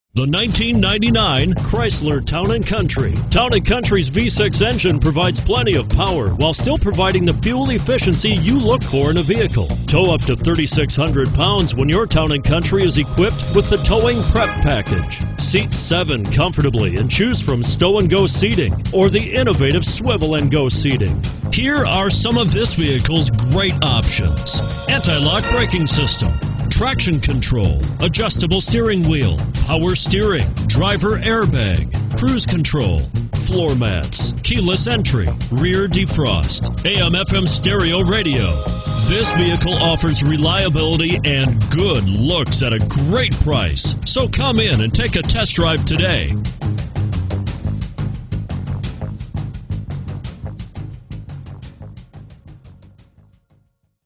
1999 CHRYSLER TOWN & COUNTRY Cherry Hill, NJ 62740
1999 CHRYSLER TOWN & COUNTRY Cherry Hill, NJ
Stock# 62740

Pick up this Green 1999 Chrysler Town & Country, available today. Featuring Automatic transmission, it has 161610 miles. This could be the one you've been searching for.